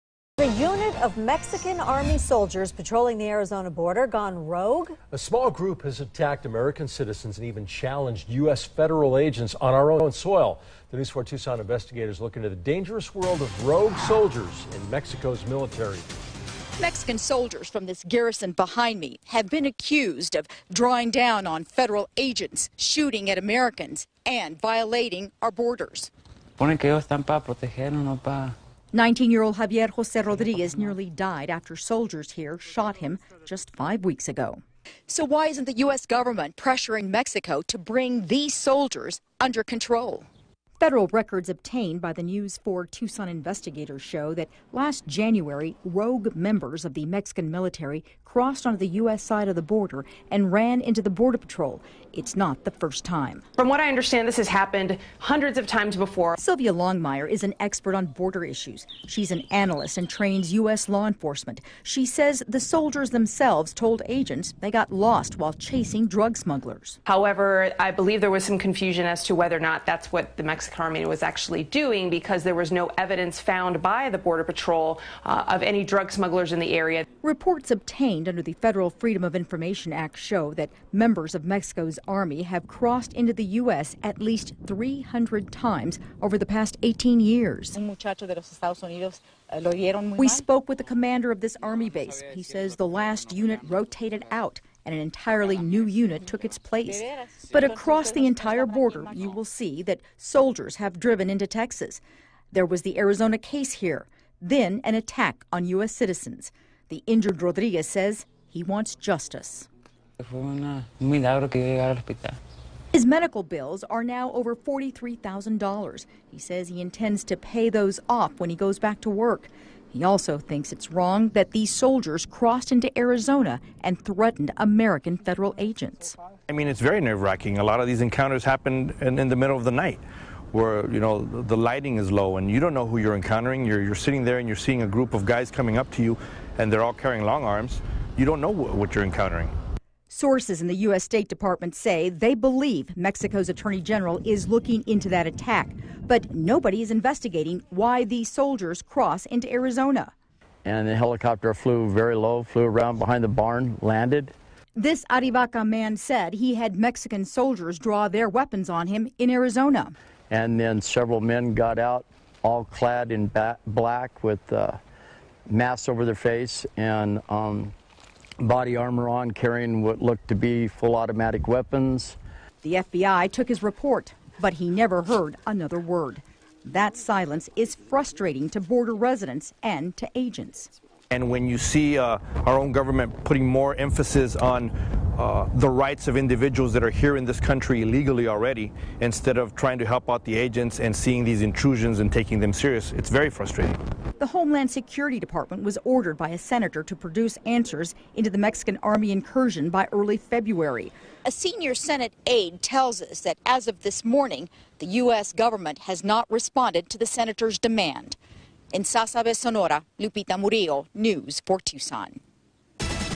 Mexican Troops Invading Arizona Border Shooting American Citizens Video
How is this not all over main stream media?  When another country comes into our country and puts weapons to our citizens.. its called an invasion... is this the cartels sending them in doing their bidding?